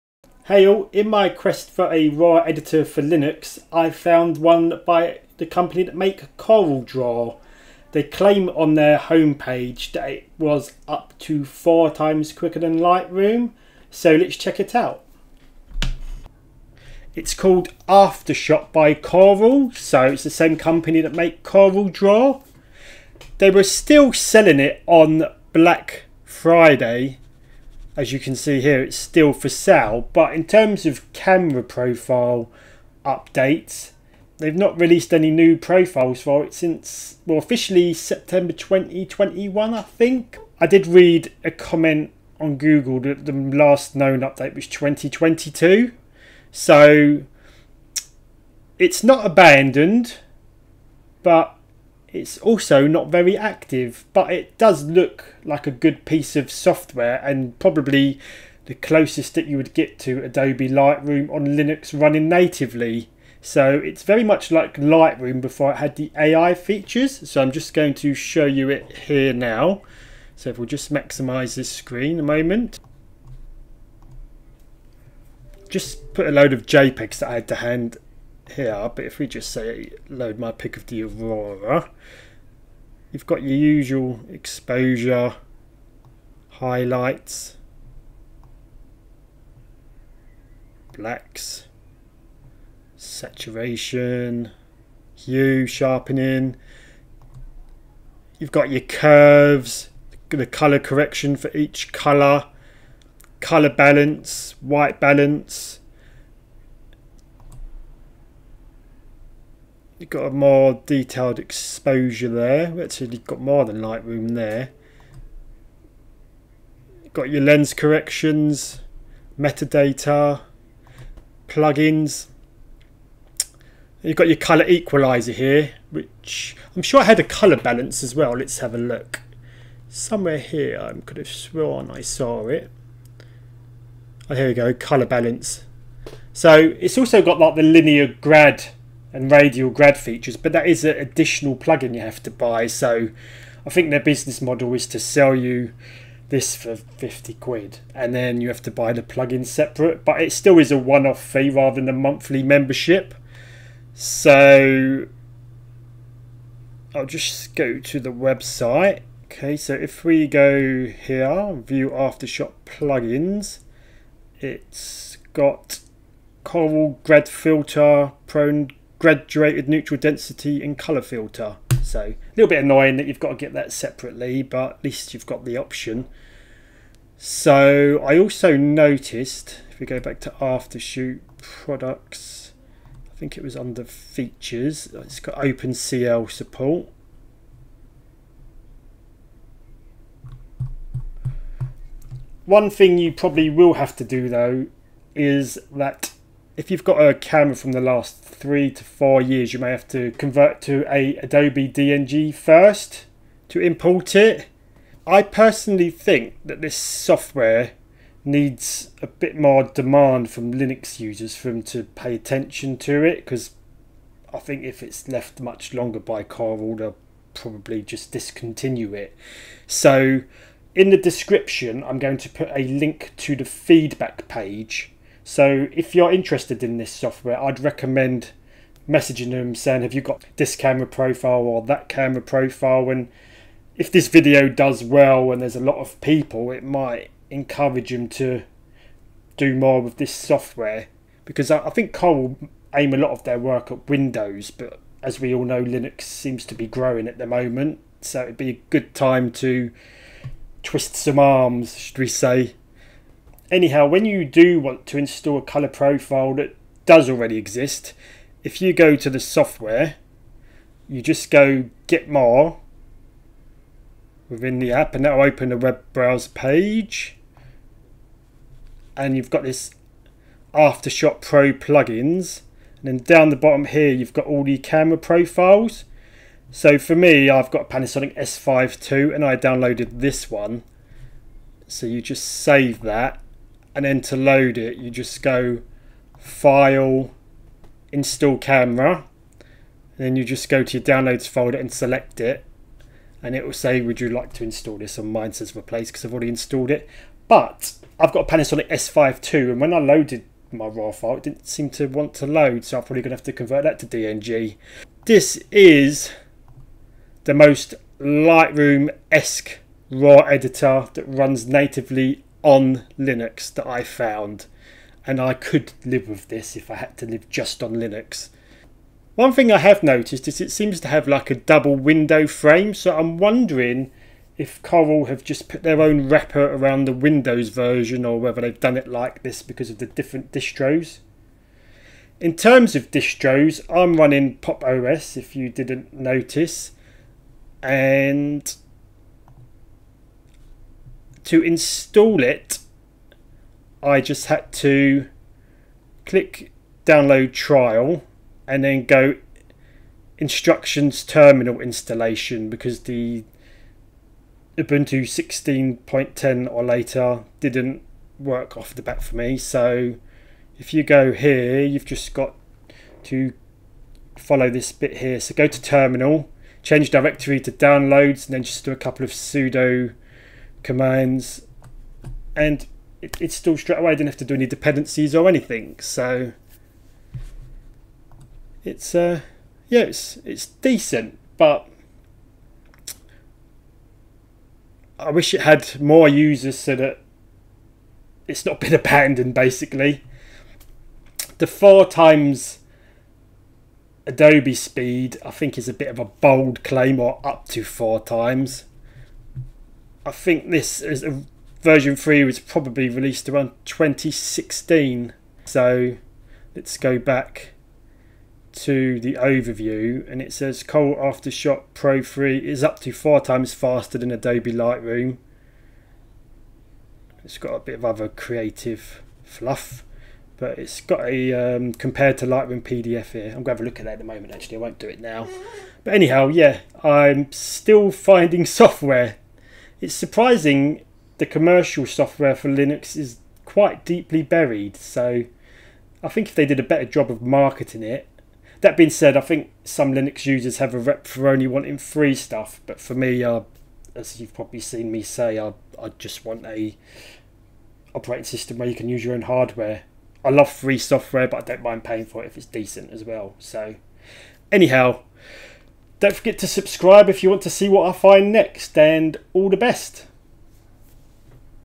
"Up to 4 x faster than Lightroom" Linux Raw editor by Corel. Bye Bye Darktable?
In this video I test the Linux release of Aftershot Pro by Corel. I noticed that it was still for sale by Corel on black Friday yet it hasn't been updated with camera profiles since 2022 so it is slowly becoming legacy.

Anyhow, lets change that! I think we should all submit feedback for the latest camera profiles and renew the demand. It seems to be in a limbo state where it hasn't quite been abandoned but it's also not being actively maintained. I think they're missing an opportunity here, Linux is crying for a Raw editor that's as simple as Lightroom.

This is the only commercial raw editor that I've seen on Linux and it has a very similar feel to Lightroom prior to AI. The linear grad tool is a separate purchased plugin though which is a tad annoying.